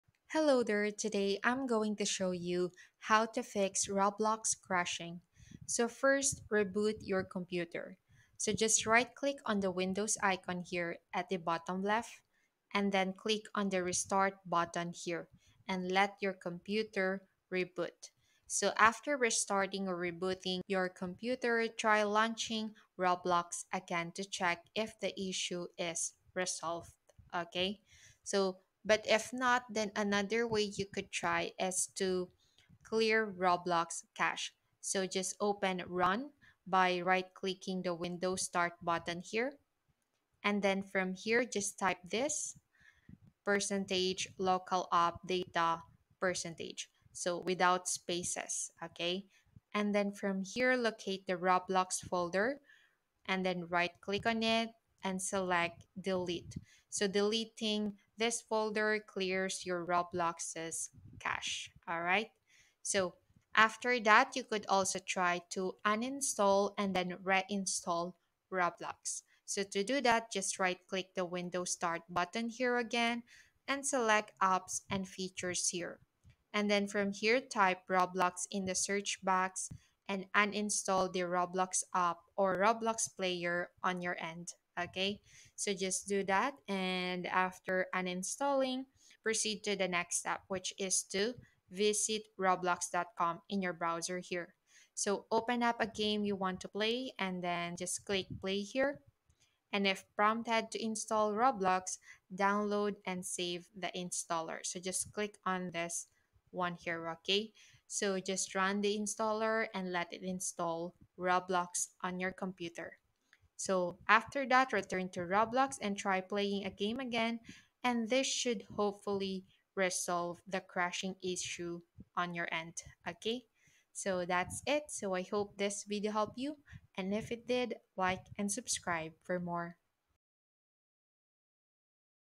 How To Fix Roblox Crashing (Easy Fix)
Is your Roblox adventure turning into a crash landing? 🚀😱 Don't worry, we've got the ultimate guide to fix those unexpected errors and keep your gaming journey smooth and glitch-free!

In this video, we'll dive into quick and effective solutions to tackle the notorious Roblox crashing issue. Whether you're on PC or facing unexpected errors, we've got the hacks to get you back in the game.